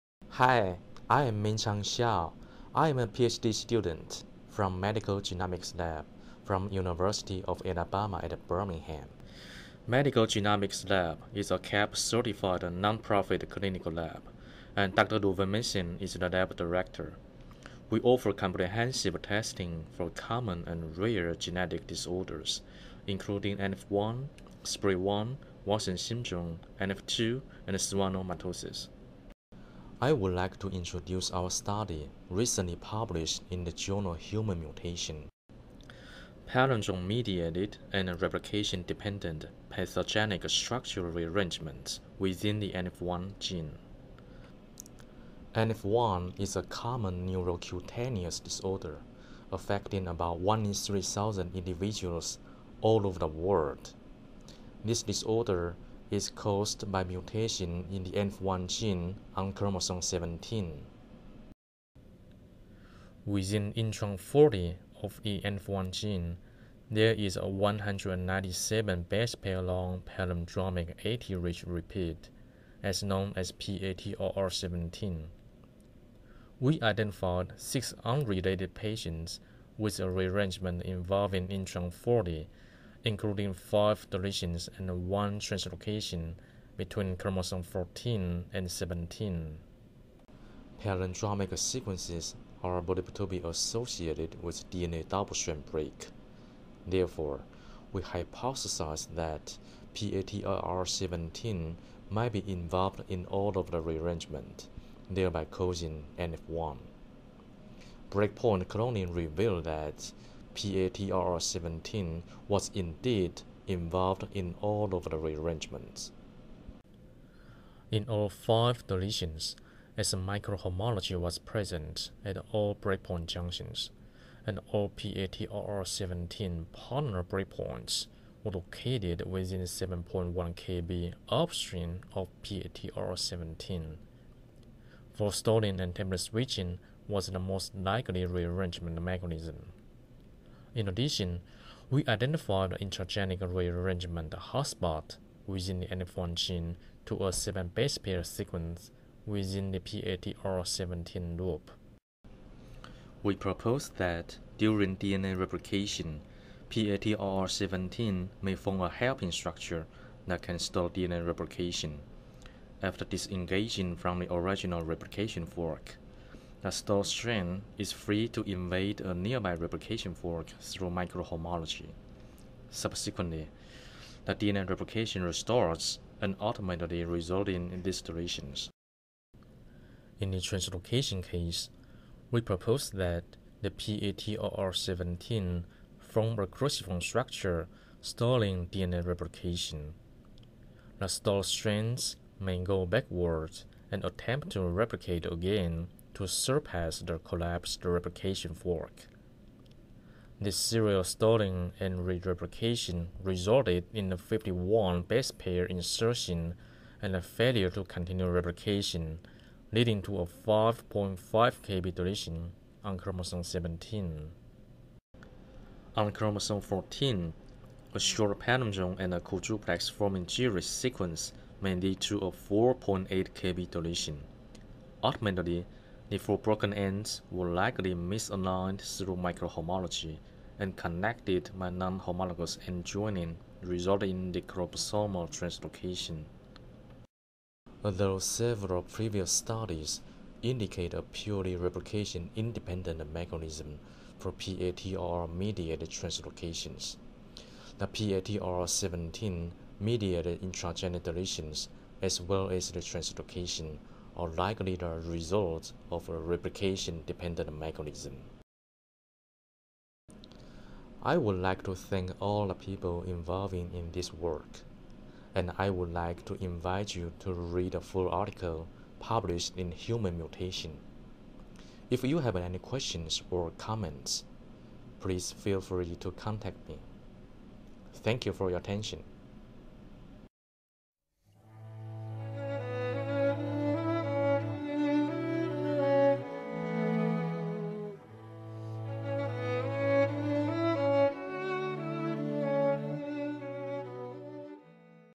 Human Mutation - Meng-Chang Hsiao
Video Highlight: Author, Meng-Chang Hsiao on his recently published Human Mutation paper entitled "Palindrome-Mediated and Replication-Dependent Pathogenic Structural Rearrangements within the NF1 Gene".